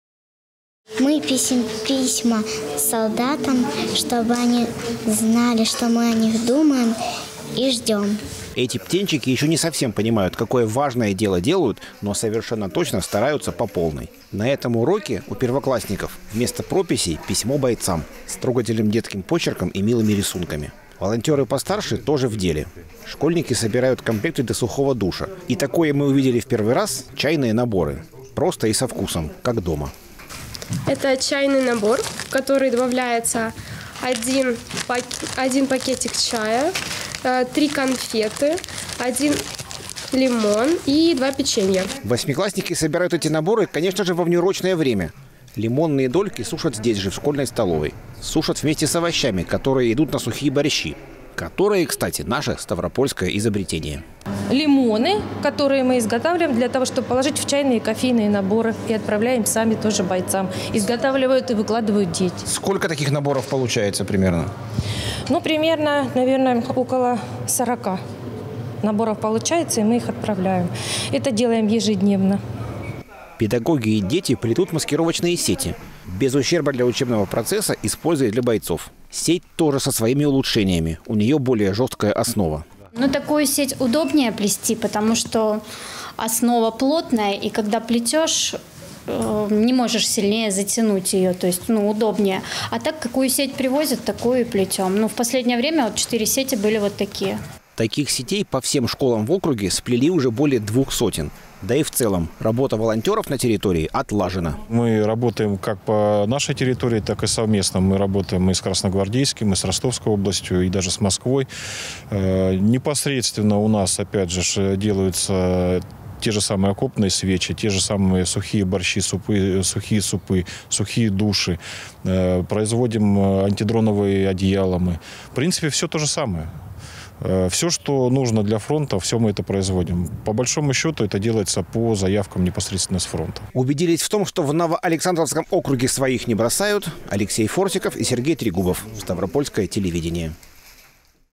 В Новоалександровском округе действуют школьные волонтёрские центры
Во всех школах Новоалександровского округа организовать работа детских волонтёрских центров.
Вместо прописи у первоклашек - письмо солдату, во время школьной перемены - сбор гуманитарных посылок. А учителя - плетут маскировочные сети.

Волонтёры постарше - тоже в деле. Школьники собирают комплекты для сухого душа и чайные наборы. Просто и со вкусом как дома. Восьмиклассники собирают эти наборы во внеурочное время, конечно же. Лимонные дольки, сушат здесь же, в школьной столовой. Сушат вместе с овощами, которые идут на сухие борщи, которые, кстати, наше, Ставропольское изобретение.

Работа волонтёров на территории отлажена, поделился глава территории Эдуард Колтунов.
“Мы работаем как по нашей территории, так и совместно - с Красногвардейским, с Ростовской областью, даже с Москвой. Непосредственно у нас делаются те же самые окопные свечи, сухие супы, сухие души, производим антидроновые одеяла. Всё, что нужно для фронта - все мы производим”

В волонтёрской деятельности на территории Новоалександровска задействованы более 5000 местных жителей, подчеркнул Эдуард Колтунов.